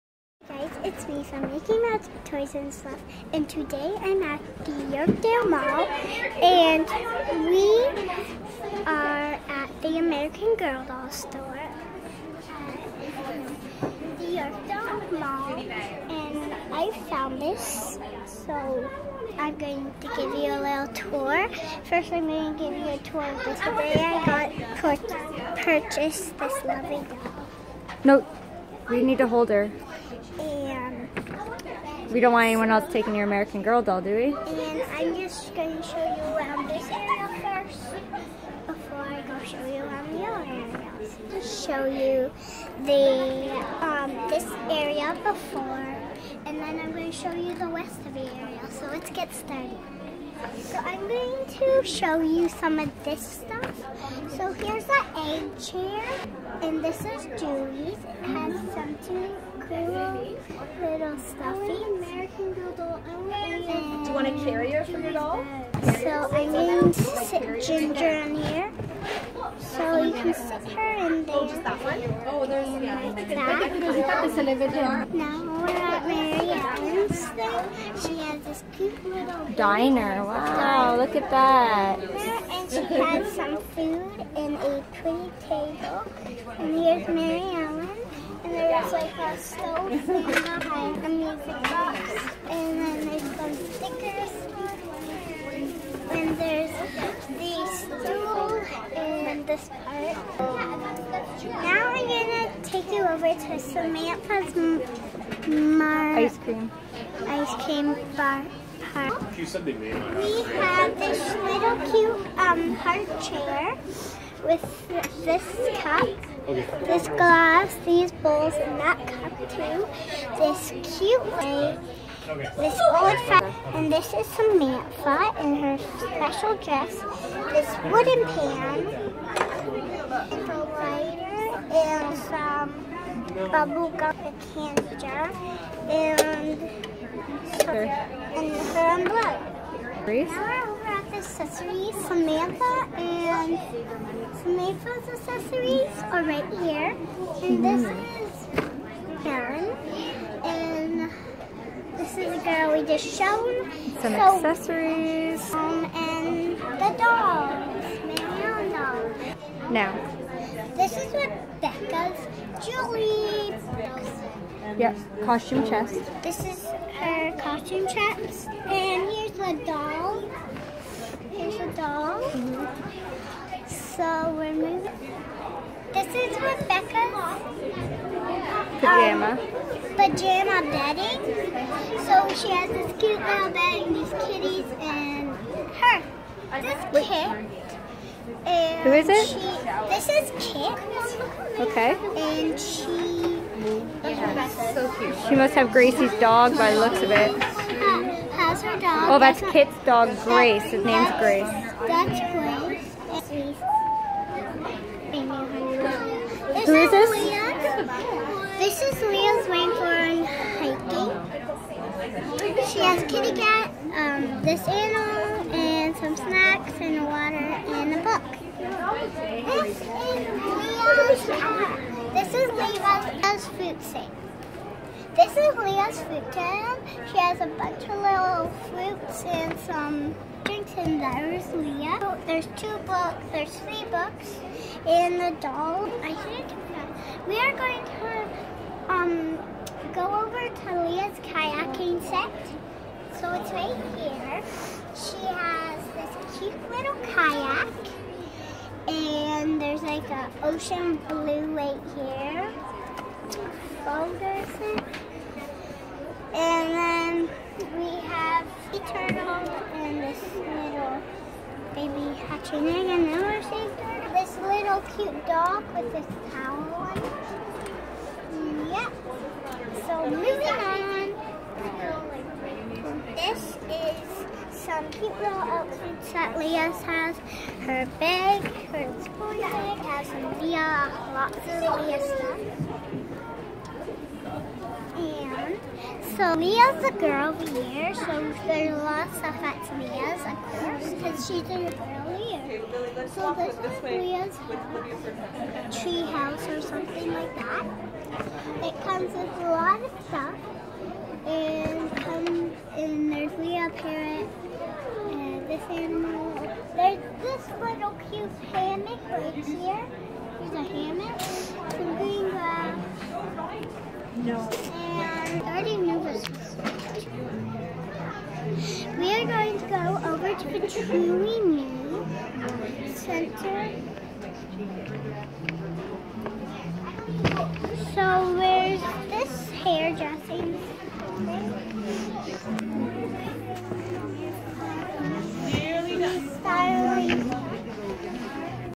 American Girls store Tour
Princess Was rather shy to do her first Video of the American  dolls  store she has wanted to make a video  of the store for so long  it was really busy so bare with her and the noise in the back ground!